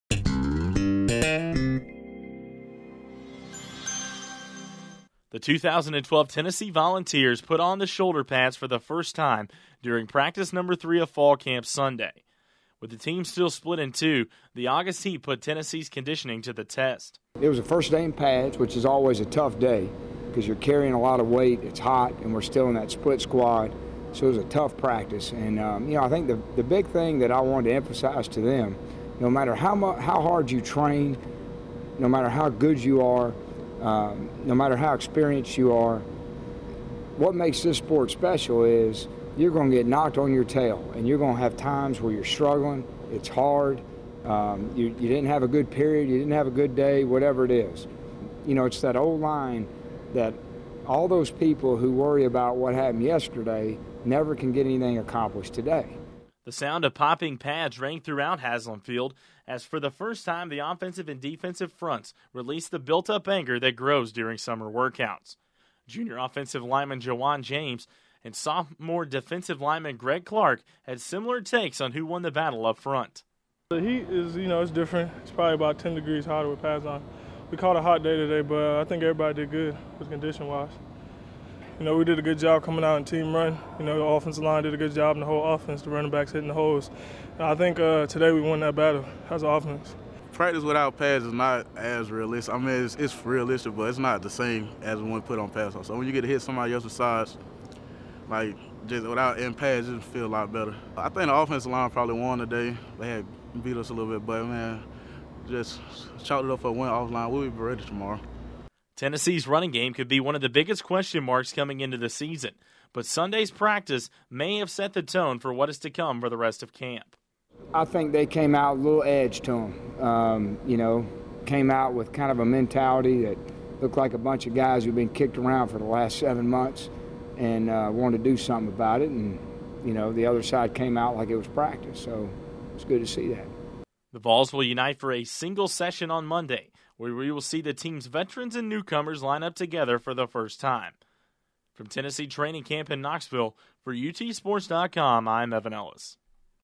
Vol Report August 5
The Vols take to Haslam Field wearing pads for the first time this Fall Camp.